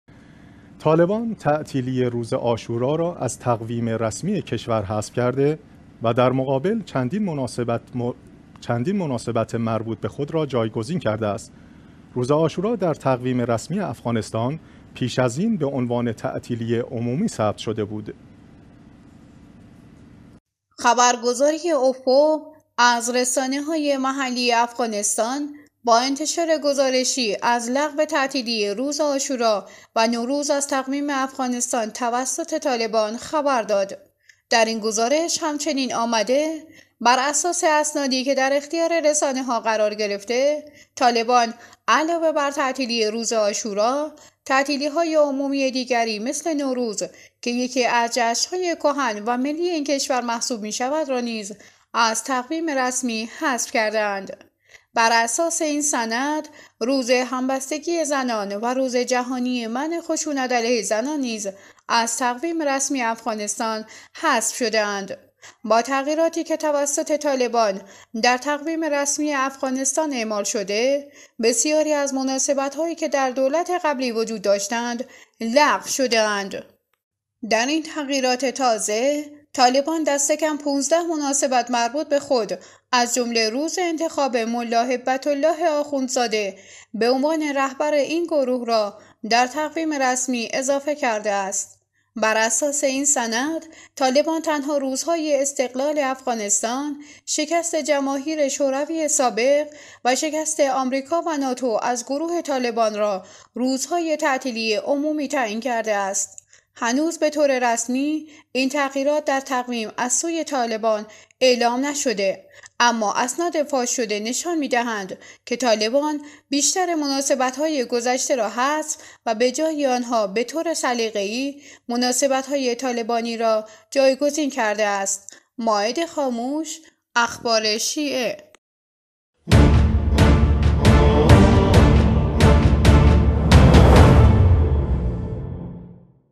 لغو تعطیلی عاشورا و نوروز از تقویم افغانستان توسط طالبان-جزئیات بیشتر در گزارش مائده خاموش
طالبان، تعطیلی روز عاشورا را از تقویم رسمی کشور حذف کرده و در مقابل چندین مناسبت مربوط به خود را جایگزین کرده است.

روز عاشورا در تقویم رسمی افغانستان پیش از این به عنوان تعطیلی عمومی ثبت شده ‌بود.

خبرگزاری افق، از رسانه های محلی افغانستان با انتشار گزارشی از لغو تعطیلی روز عاشورا و نوروز از تقویم افغانستان توسط طالبان خبر داد.

در این گزارش همچنین آمده: بر اساس اسنادی که در اختیار رسانه‌ها قرار گرفته، طالبان علاوه بر تعطیلی روز عاشورا، تعطیلی‌های عمومی دیگری مثل نوروز که یکی از جشن‌های کهن و ملی این کشور محسوب می‌شود را نیز از تقویم رسمی حذف کرده است.

بر اساس این سند، روز همبستگی زنان و روز جهانی منع خشونت علیه زنان نیز از تقویم رسمی افغانستان حذف شده‌اند.

با تغییراتی که توسط طالبان در تقویم رسمی افغانستان اعمال شده، بسیاری از مناسبت‌هایی که در دولت قبلی وجود داشتند، لغو شده‌اند.

در این تغییرات تازه، طالبان دست کم ۱۵ مناسبت مربوط به خود از جمله روز انتخاب "ملا هبت الله آخوندزاده" به عنوان رهبر این گروه را در تقویم رسمی اضافه کرده است.

بر اساس این سند، طالبان تنها روزهای استقلال افغانستان، شکست جماهیر شوروی سابق و شکست آمریکا و ناتو از گروه طالبان را روزهای تعطیلی عمومی تعیین کرده است.

هنوز به طور رسمی این تغییرات در تقویم از سوی طالبان اعلام نشده اما اسناد فاش‌شده نشان می‌دهند که طالبان بیش‌تر مناسبت‌های گذشته را حذف و به جای آن‌ها به طور سلیقه‌ای مناسبت‌های طالبانی را جایگزین کرده است.